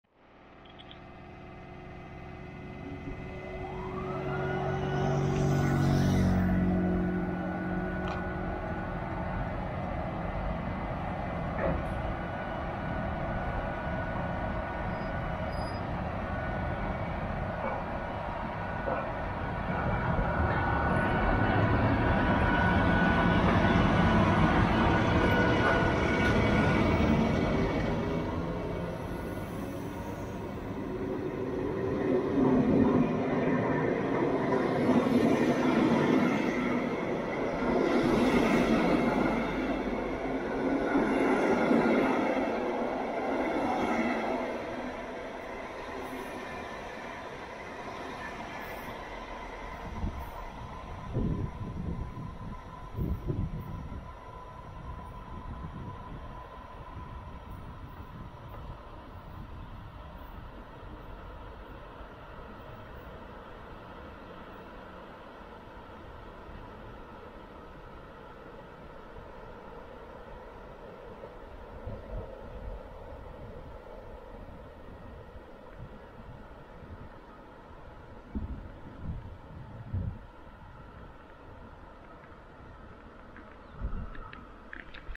H-START 433 227 (V43 3227) indul a Tópart IC-vel Balatonszárszóról, 2022. 09. 04. [4K]
V43 3227 indul a 845 számú Nagykanizsa - Budapest-Déli pu. Tópart IC-vel Balatonszárszóról, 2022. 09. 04-én.
H-START 433 227 departs from Balatonszárszó, with number 845 Nagykanizsa (Hungary) - Budapest-Déli Station Tópart (Lake Shore) InterCity-train, at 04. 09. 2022.
Video.: Petrás Bence